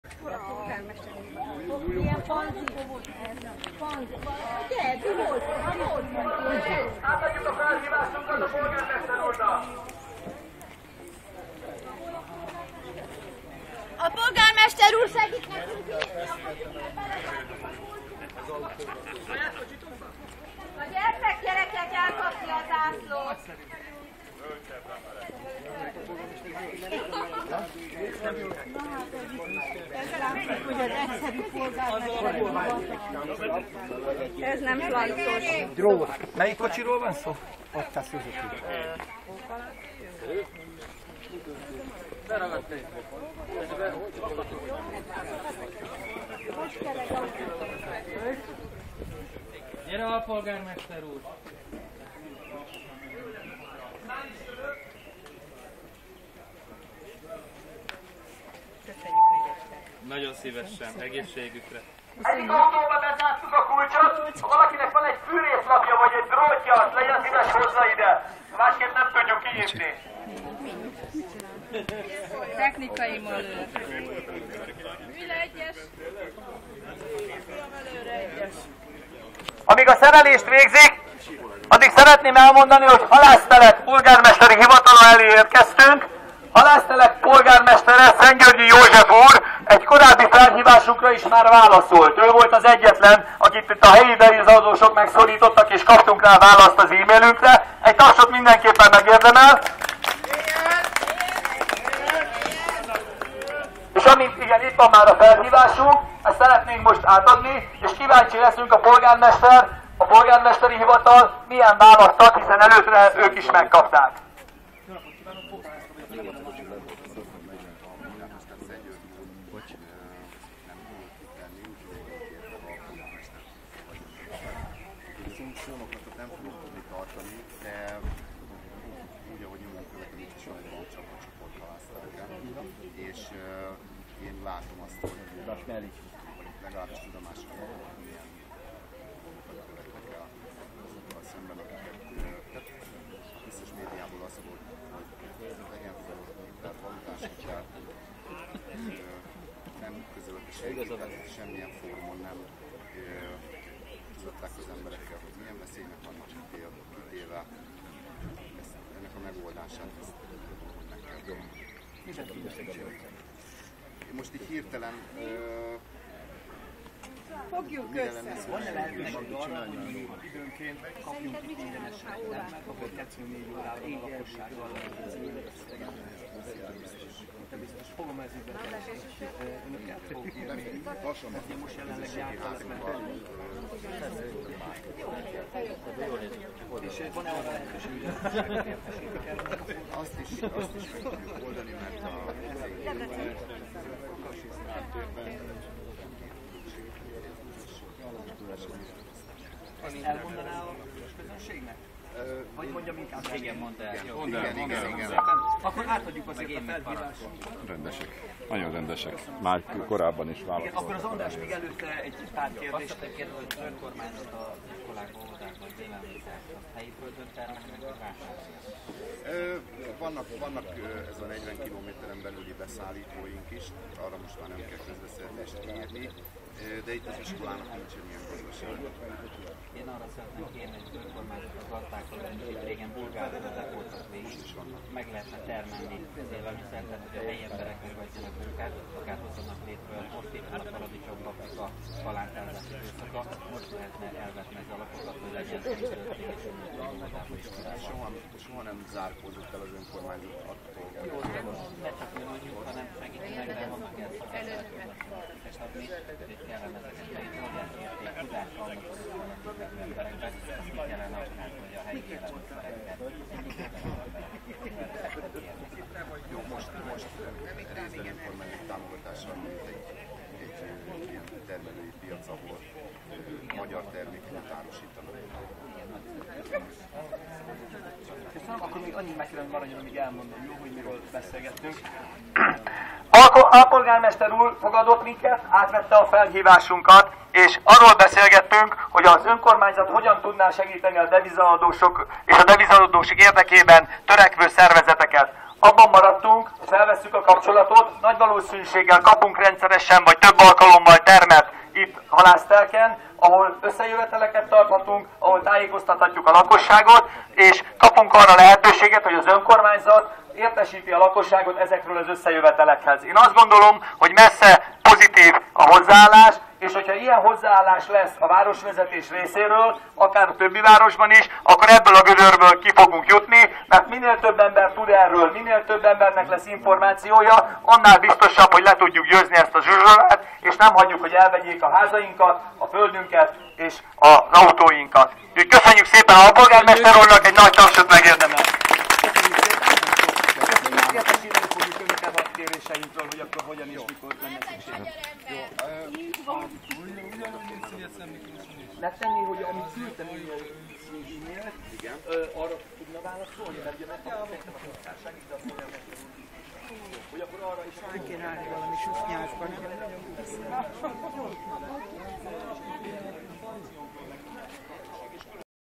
2013.05.29 Szigetszentmiklós és környéke (autós demonstráció) - Halásztelek Polgármesteri Hivatal
Halásztelek Polgármesteri Hivatala előtt átadtuk a petíciónkat a polgármester helyettes úrnak:
Felhívnám a figyelmet a kulcsmondatokra!
"...TUDOMÁSUNK VAN RÓLA..."
"AZ ÖSSZES MÉDIÁBÓL AZ FOLYT, HOGY VEGYENEK FEL HITELT..." "...SEMMILYEN FÓRUMON NEM TUDATTÁKAZ EMBEREKET MILYEN VESZÉLYEK VANNAK KITÉVE..."

Taksonyban Kreisz László polgármester úr hasonlóan nyilatkozott, ő volt az, aki mint a hatalom jelenlegi megtestesítője - a nép választott képviselője -, először mondta ki "DEVIZACSAPDA" kifejezést "...ISMERJÜK EZT A PROBLÉMÁT..."

A DEVIZAHITEL HIBÁS TERMÉK! A DEVIZAHITEL UZSORAHITEL! NE FIZESS UZSORÁT! A BANK HALÁLA, A PÉNZ HIÁNYA! NE FIZESS TOVÁBB!